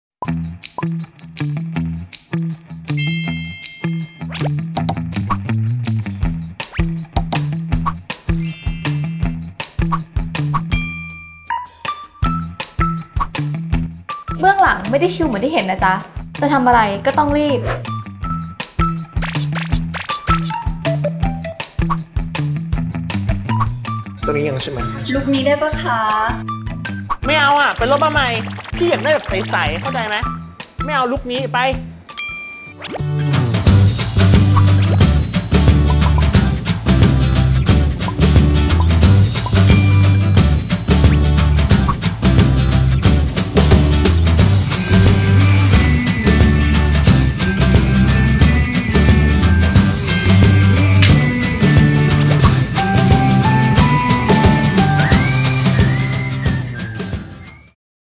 ทริคเด็ด เคลียร์ผิวใส รวบตึงทุกขั้นตอน ด้วย siste'o oxy bubble cleanser | SistaCafe
*Playlist* ออกกำลังกาย ลดน้ำหนัก

ช่องทางอื่นๆ ของ SistaCafe